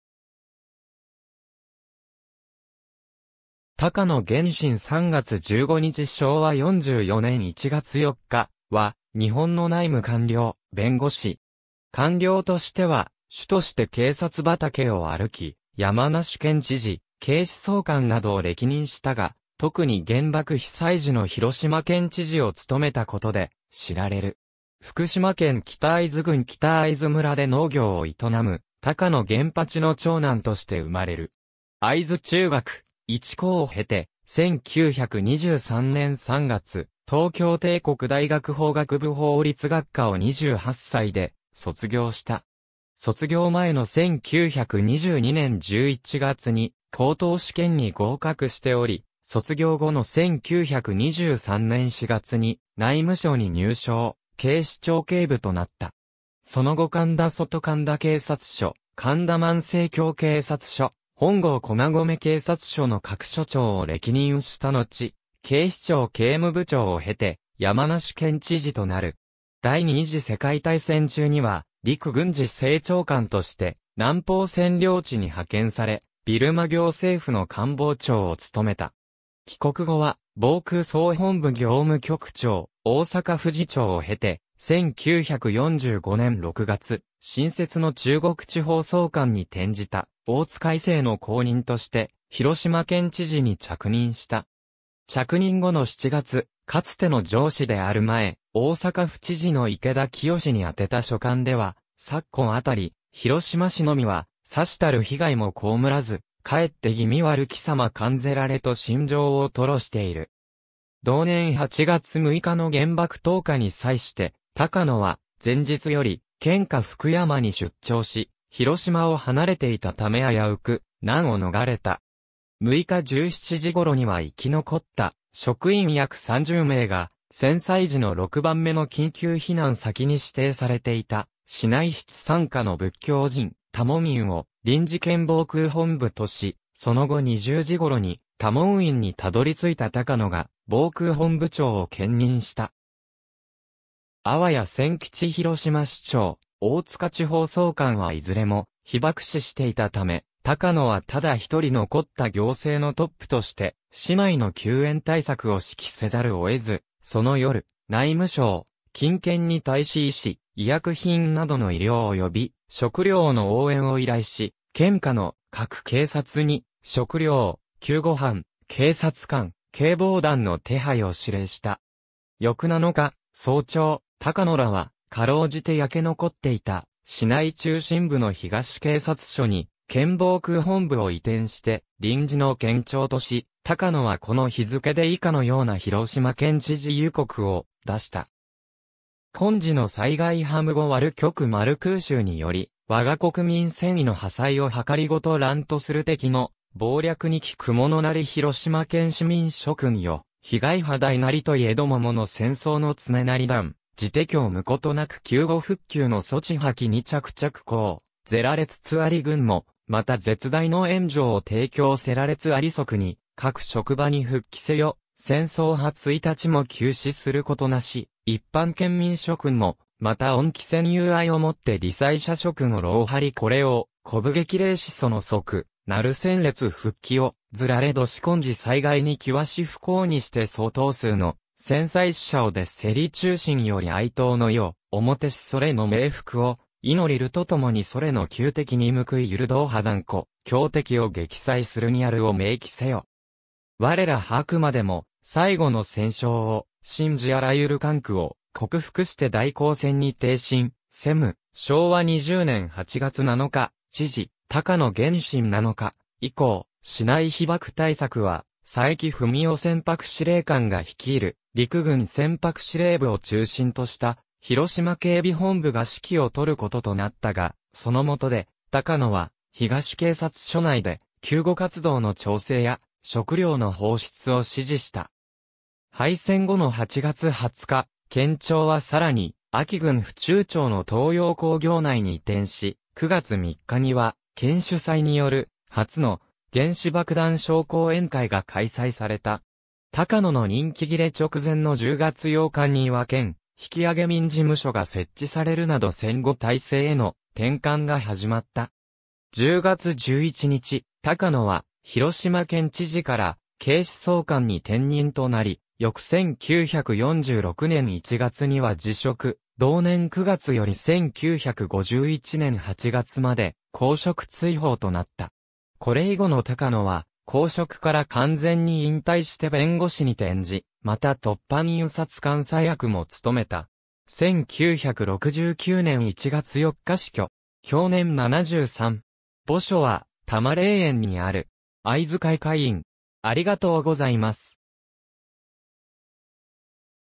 高野源進
高野 源進（たかの げんしん、明治28年（1895年）3月15日 - 昭和44年（1969年）1月4日）は、日本の内務官僚、弁護士。
官僚としては主として警察畑を歩き山梨県知事・警視総監などを歴任したが、特に原爆被災時の（官選）広島県知事を務めたことで知られる。
福島県北会津郡北会津村で農業を営む高野源八の長男として生まれる。
会津中学・一高を経て、1923年3月東京帝国大学法学部法律学科（独法学）を28歳で卒業した。
卒業前の1922年11月に高等試験に合格しており、卒業後の1923年4月に内務省に入省、警視庁警部（警務部警衛課）となった。
その後神田外神田警察署・神田万世橋警察署・本郷駒込警察署の各署長を歴任したのち、警視庁警務部長（1939年9月 - 1941年1月）を経て、山梨県知事となる（1941年1月 - 1942年7月）。
第二次世界大戦（太平洋戦争）中には陸軍司政長官（1942年7月7日発令）として南方（東南アジア）占領地に派遣され、ビルマ行政府（ビルマ国発足以前の行政組織）の官房長を務めた（1942年8月 - 1943年8月）。
帰国後は防空総本部業務局長・大阪府次長を経て、1945年6月、新設の中国地方総監に転じた大塚惟精の後任として広島県知事に着任した。
着任後の7月、かつての上司である前・大阪府知事の池田清に宛てた書簡では「昨今当広島市のみはさしたる（空襲の）被害も蒙らず、却って気味悪き様感ぜられ」と真情を吐露している。
同年8月6日の原爆投下に際して高野は、前日より県下福山に出張し広島を離れていたため危うく難を逃れた（ただし広島に残された高野の家族は被爆し、夫人のシナは死去した）。
6日17時頃には生き残った職員約30名が、戦災時の6番目の緊急避難先に指定されていた市内比治山下の仏教寺院・多聞院を「臨時県防空本部」とし、その後20時頃に多聞院にたどり着いた高野が防空本部長を兼任した（なお高野の帰広は18時半で、その後県防空本部を設置したとの説もある）。
粟屋仙吉広島市長・大塚地方総監はいずれも被爆死していたため、高野はただ一人残った行政のトップとして市内の救援対策を指揮せざるをえず、その夜、内務省・近県に対し医師・医薬品などの医療および食糧の応援を依頼し、県下の各警察に食糧・救護班・警察官・警防団の手配を指令した。
翌7日早朝、高野らはかろうじて焼け残っていた市内中心部の東警察署（現在の広島銀行銀山町支店に所在）に県防空本部を移転して臨時の県庁とし、高野はこの日付で以下のような広島県知事諭告を出した。
今次ノ災害ハ惨悪極マル空襲ニヨリ我国民戦意ノ破砕ヲ謀ラントスル敵ノ暴略ニ基クモノナリ広島県市民諸君ヨ 被害ハ大ナリト雖モ之戦争ノ常ナリ　断ジテ怯ムコトナク救護復旧ノ措置ハ既ニ着々講ゼラレツツアリ　軍モ亦絶大ノ援助ヲ提供セラレツゝアリ　速ニ各職場ニ復帰セ ヨ、戦争ハ一日モ休止スルコトナシ　一般県民諸君モ亦温キ戦友愛ヲ以テ罹災者諸君ヲ労ハリ　之ヲ鼓舞激励シ其速ナル戦列復帰ヲ図ラレ度シ　今次災害二際シ不幸ニシテ相当数ノ戦災死者ヲ出セリ　衷心ヨリ哀悼ノ意ヲ表シ其ノ冥福ヲ祈ルト共二其ノ仇敵ニ酬ユル道ハ断乎驕敵ヲ撃砕スルニアルヲ銘記セヨ
我等ハ飽迄モ最後ノ戦勝ヲ信ジアラユル艱苦ヲ克服シテ大皇戦ニ挺身セム — 昭和二十年八月七日 知事 高野源進 7日以降、市内被爆対策は、佐伯文郎船舶司令官が率いる陸軍船舶司令部（いわゆる「暁部隊」）を中心とした「広島警備本部」が指揮をとることとなったが、そのもとで高野は東警察署内で救護活動の調整や食糧の放出を指示した。
敗戦後の8月20日、県庁はさらに安芸郡府中町の東洋工業内に移転し、9月3日には県主催による初の「原子爆弾症講演会」が開催された。
高野の任期切れ直前の10月8日には県引揚民事務所が設置されるなど戦後体制への転換が始まった。
10月11日、高野は広島県知事から警視総監に転任となり、翌1946年1月には辞職、同年9月より1951年8月まで公職追放となった。
これ以後の高野は公職から完全に引退して弁護士に転じ、また凸版印刷監査役も務めた。
1969年1月4日死去。
享年73。
墓所は多磨霊園にある。
会津会会員。